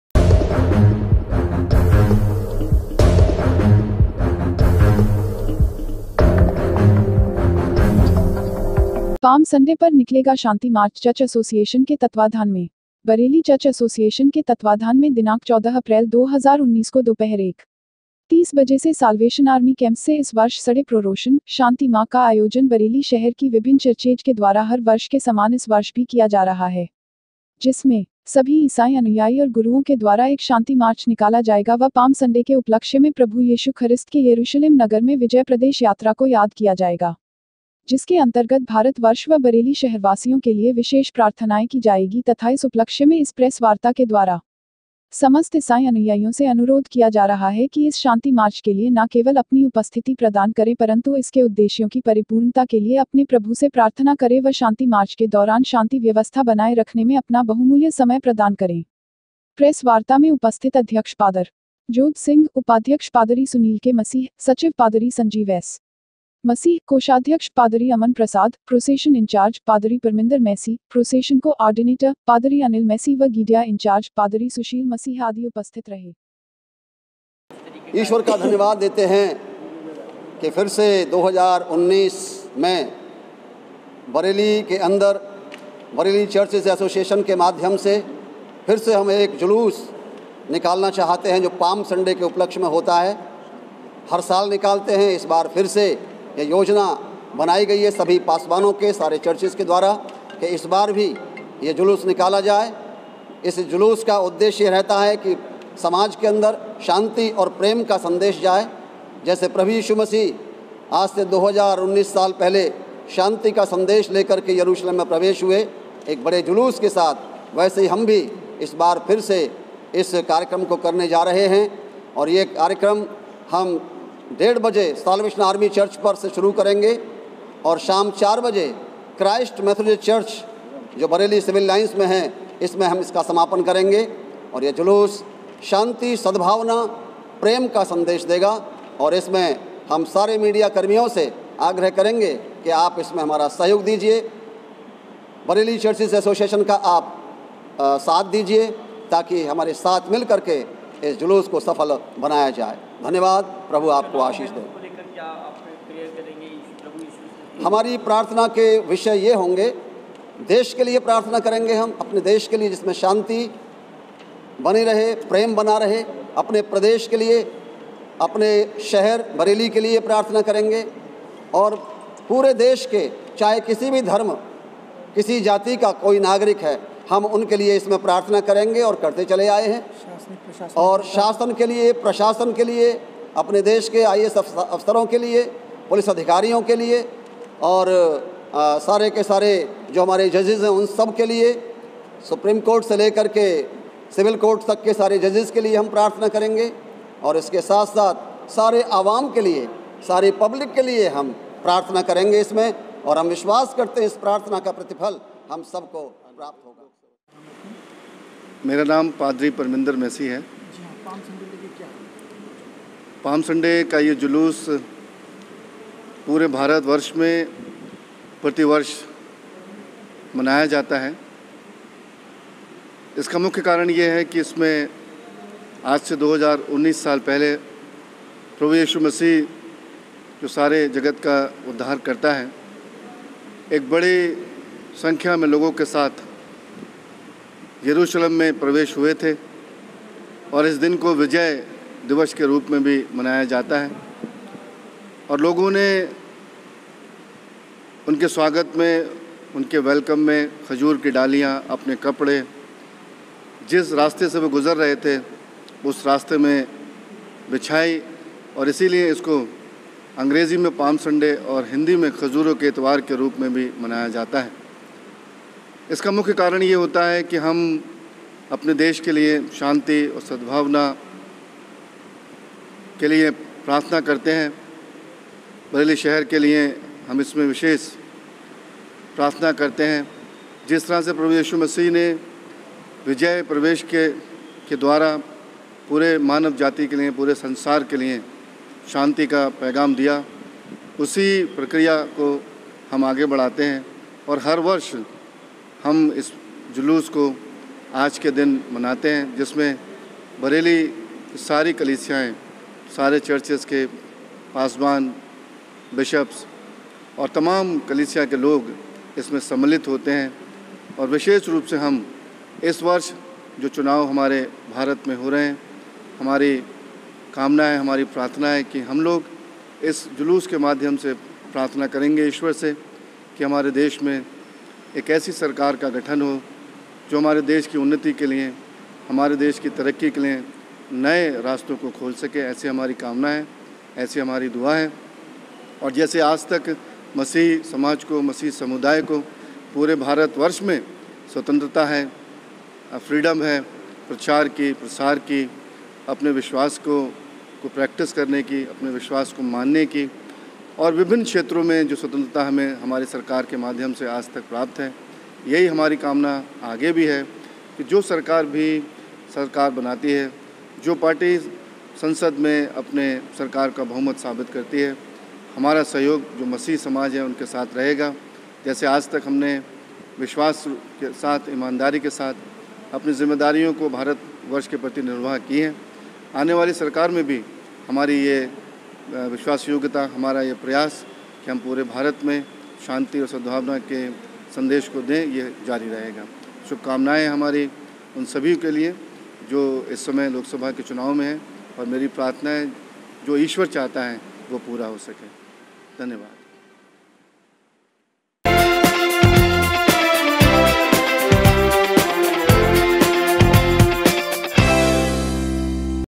Bareilly News : पाम संडे पर निकलेगा शांति मार्च चर्च एसोसिएशन के नेतृत्व में
बरेली चर्च एसोसिएशन के तत्वाधान में दिनांक 14 अप्रैल 2019 को दोपहर 1 . 30 बजे से सालवेशन आर्मी कैम्पससे इस वर्ष  सड़े प्रोरोशन ( शांति मा ) का आयोजन बरेली शहर की विभिन्न चर्चेज के द्वारा हर वर्ष के समान इस वर्ष भी किया जा रहा है । जिसमें सभी ईसाई अनुयायी औऱ गुरूओं के द्वारा एक शांति मार्च निकाला जायेगा व पाम संडे के उपलक्ष्य में प्रभु येशु ख्रिस्त के येरूशलेम नगर में विजय प्रदेश यात्रा को याद किया जायेगा । जिसके अन्तर्गत भारत वर्ष व बरेली शहरवासियों के लिए विशेष प्रार्थनायें की जायेगी तथा इस उपलक्ष्य में इस प्रेस वार्ता के द्वारा समस्त इसाई अनुयायियों से अनुरोध किया जा रहा है कि इस शांति मार्च के लिए ना केवल अपनी उपस्थिति प्रदान करें परन्तु इसके उद्देश्यों की परिपूर्णता के लिए अपने प्रभु से प्रार्थना करे व शांति मार्च के दौरान शांति व्यवस्था बनाये रखने में अपना बहुमूल्य समय प्रदान करें । प्रेस वार्ता में उपस्थित अध्यक्ष पादरी जोध सिंह , उपाध्यक्ष पादरी सुनील के मसीह , सचिव पादरी संजीव एस . मसीह , कोषाध्यक्ष - पादरी अमन प्रसाद , प्रोसेशन इन्चार्ज – पादरी परमिन्दर मैसी , प्रोसेशन को - आर्डिनेटर - पादरी अनिल मैसी व गीडिया इन्चार्ज – पादरी सुशील मसीह आदि उपस्थित रहे।